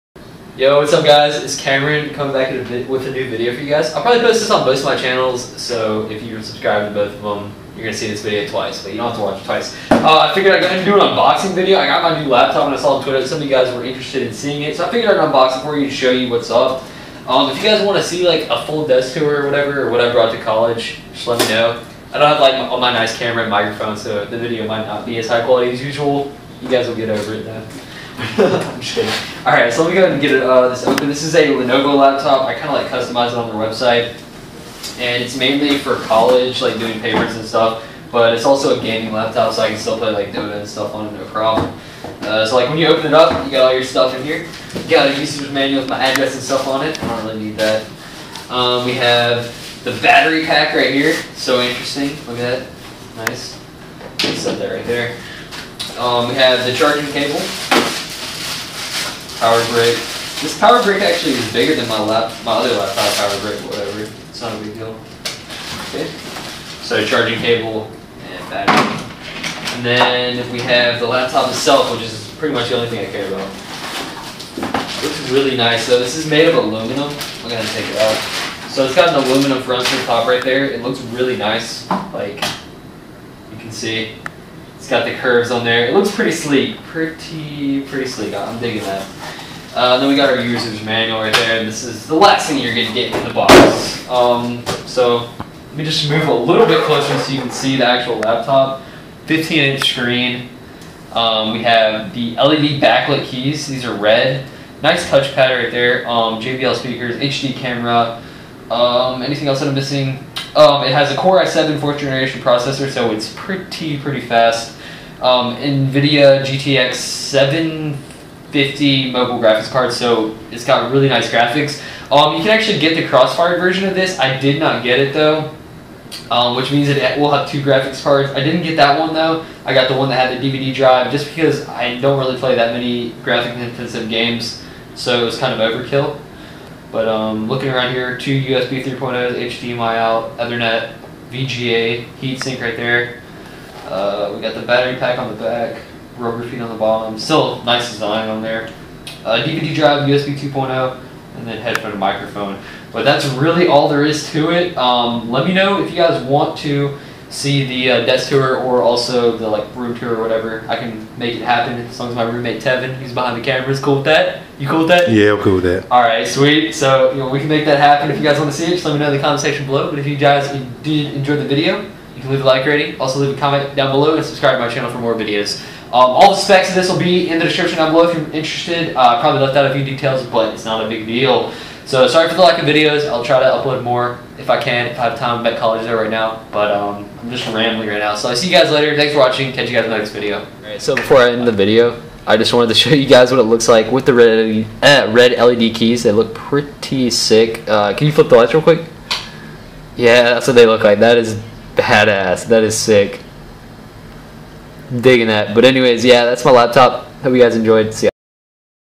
My New EPIC Gaming Laptop - Lenovo Y10p! (Unboxing
Just figured I would do a quick unboxing to show you guys my new laptop since some of you were interested! I'm at college so I have limited video making time and equipment so videos might not be too often. Sorry! :)

FULL LIST OF SPECS:

►Processor
4th Generation Intel Core i7-4700MQ Processor (2.40GHz 1600MHz 6MB)
►Operating System
Windows 8 64
►Display
15.6" FHD LED Glossy Wedge 1920x1080
►Graphics
NVIDIA GeForce GT750M GDDR5 2GB
►Memory
8.0GB PC3-12800 DDR3L SDRAM 1600 MHz
►Hard Drive
1TB 5400 RPM+24GB SSD
►Optical Device
Ultrabay SLI Graphics - NVIDIA GeForce GT750M GDDR5 2GB
►Network Card
Intel Centrino Wireless N-2230
►Bluetooth
Bluetooth Version 4.0
►Warranty
One year
►Battery
6 Cell Lithium-Ion